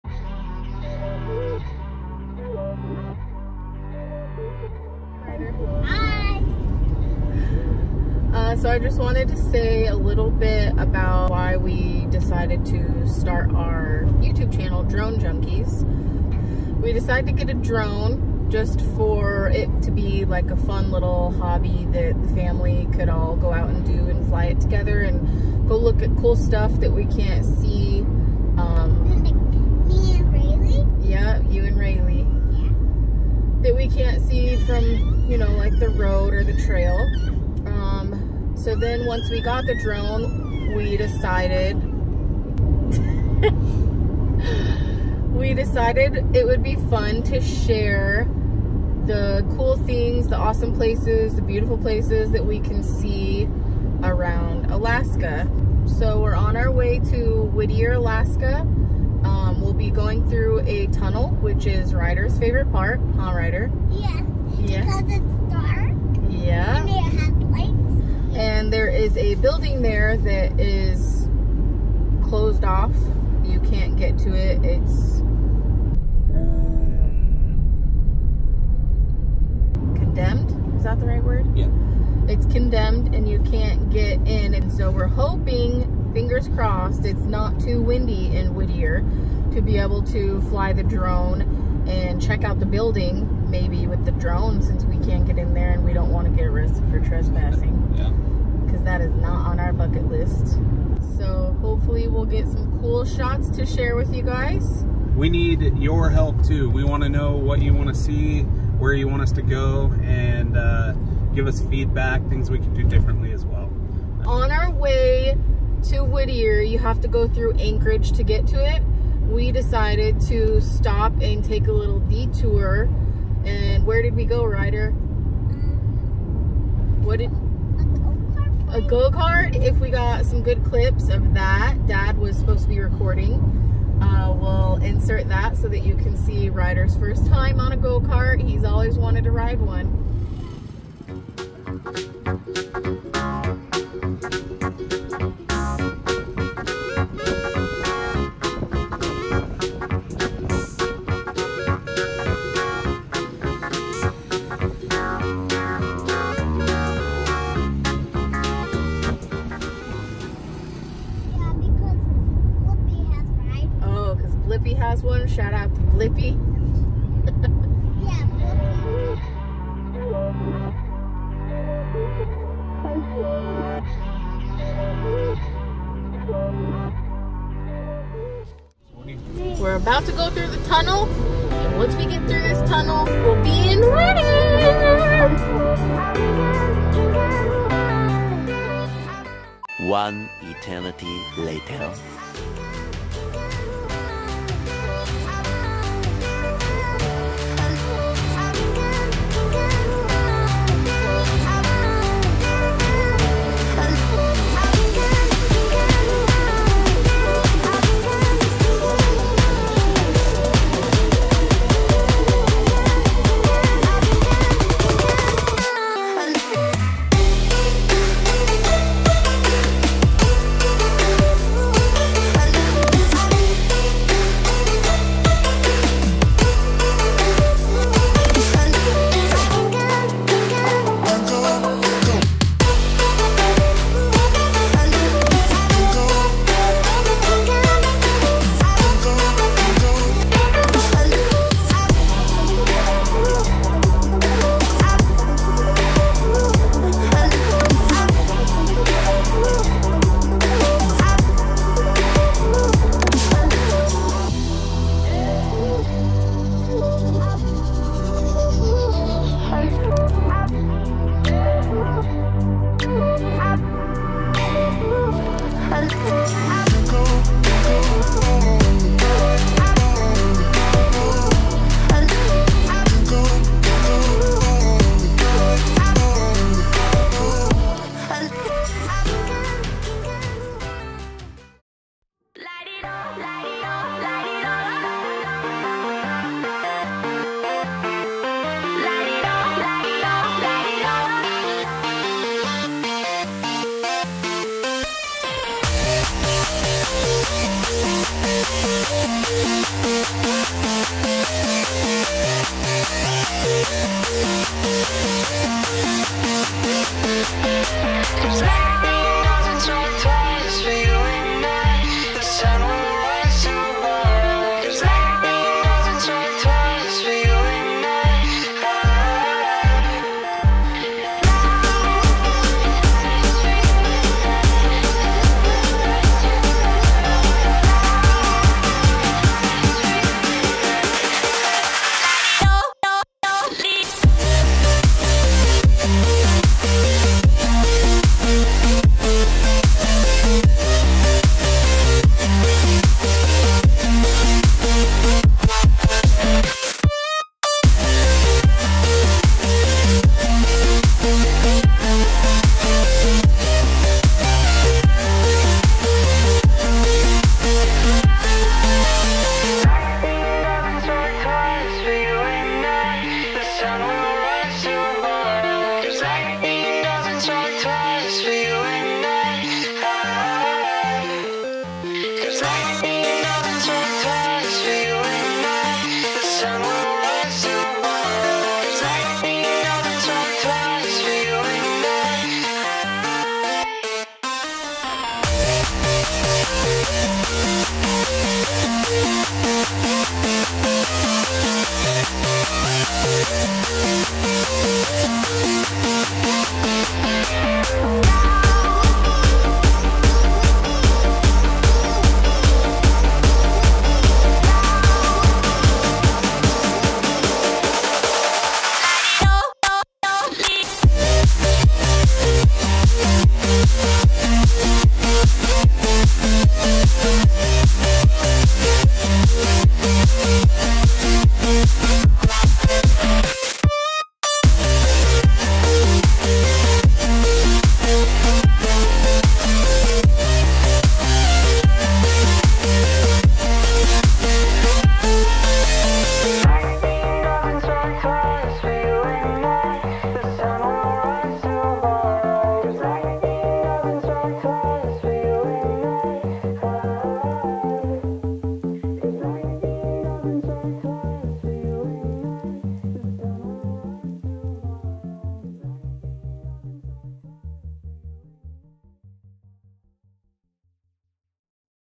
Adventure to Whittier
We took a trip to the beautiful Whittier, Alaska! We were thankfully able to catch some awesome shots of a few tourist attractions there. We had a lot of fun shooting and making this video, we hope you guys enjoy it as much as we did! Your feedback is greatly appreciated!
Drone Junkies, OUT! :)